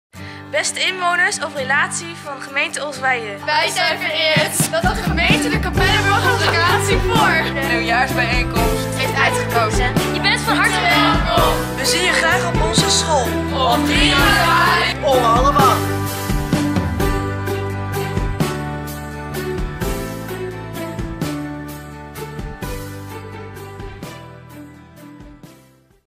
Uitnodiging nieuwjaarsreceptie Gemeente Olst-Wijhe op Capellenborg
De Gemeente Olst-Wijhe heeft onze mooie school Capellenborg gekozen om de nieuwjaarsreceptie te laten plaatsvinden op 3 januari 2017 om 19:30 uur. Jong en oud, is van harte welkom!